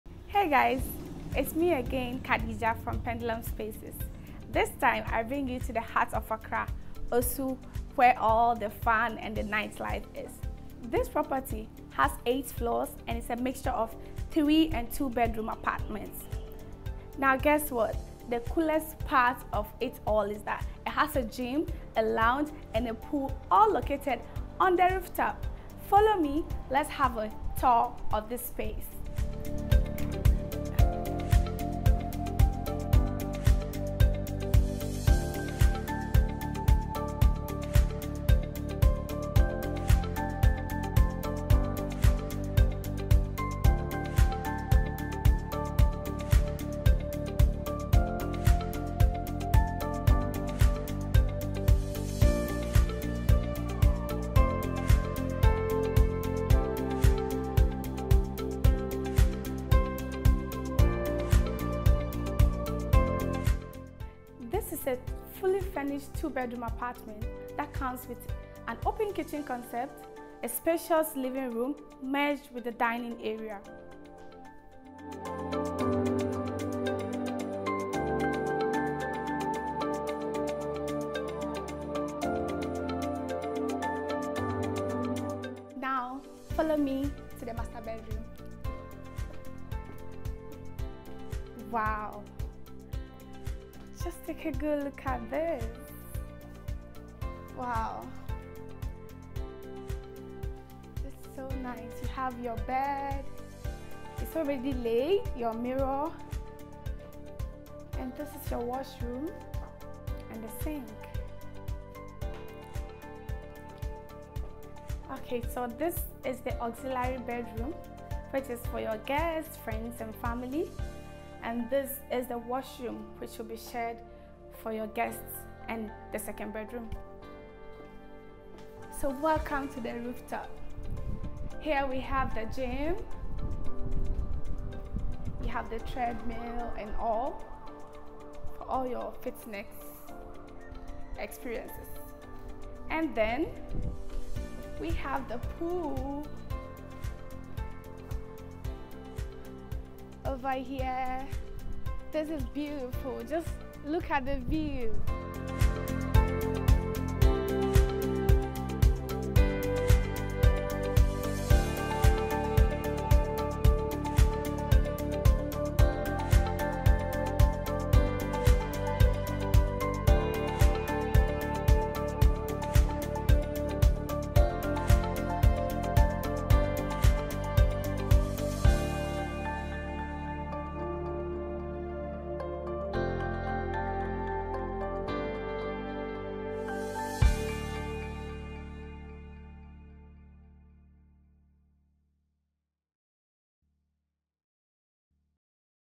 Luxury Living at its Finest: An Exclusive Tour Of Our Jaw-Dropping Listing at Osu, Accra
Sitting in one of Accra’s most popular hot spot locations, The Urban is an eight-story apartment with state-of-the-art facilities, modern fittings and also 24/7 facility management to make your stay a bliss in comfort. With the close proximity to places like banks, supermarkets, international schools, hospitals and all the fun spots of Accra, this listing Is just the perfect spot for anyone looking for that peace and quiet and yet close to all one will need to enjoy the perfect stay Accra.

Sale Price:  $330,000 - $385,000 negotiable
Rental price: $2,500 (includes service charges)
Features;
- Fully furnished units
- Spacious living and dining area
- Spacious bedrooms
- Spacious bathrooms
- Guest washrooms
- Open kitchen concept with high-end appliance fittings
- Elevator
- Rooftop pool, gym, bar and lounge.
- Basement car park.
- 24/7 security, water and facility management service.